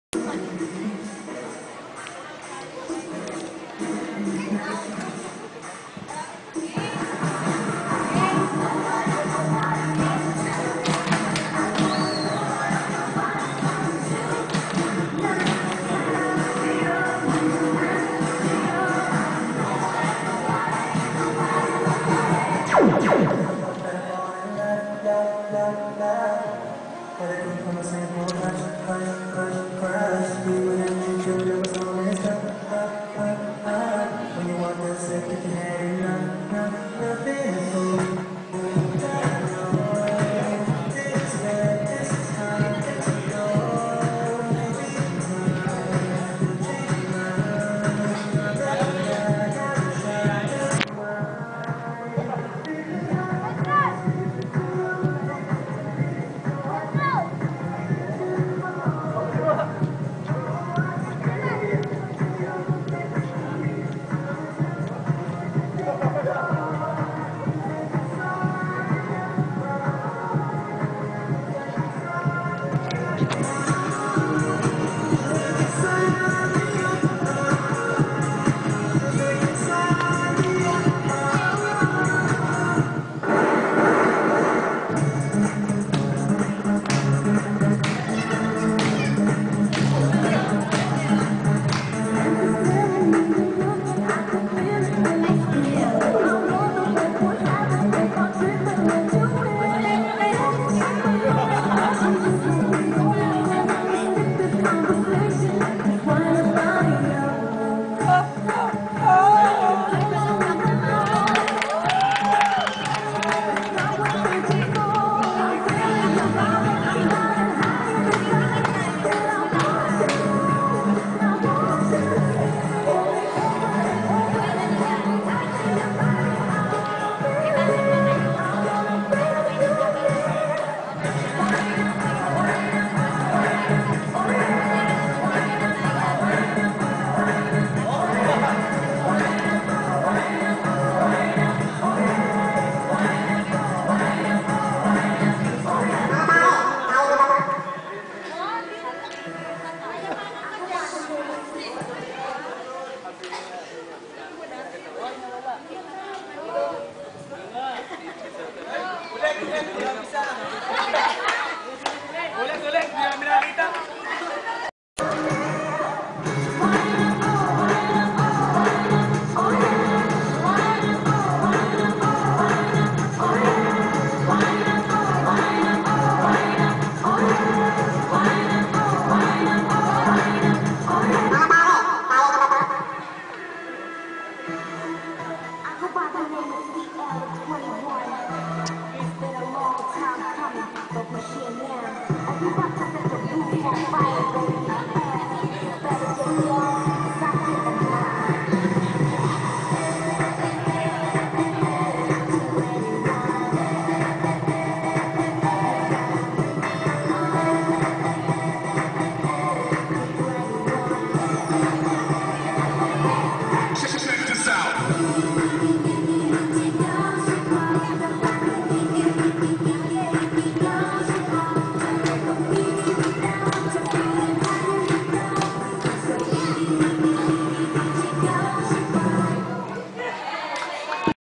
SSS Xmas Party | OJT Students
Performed by OJT of SSS Calamba Branch Christmas Party 2010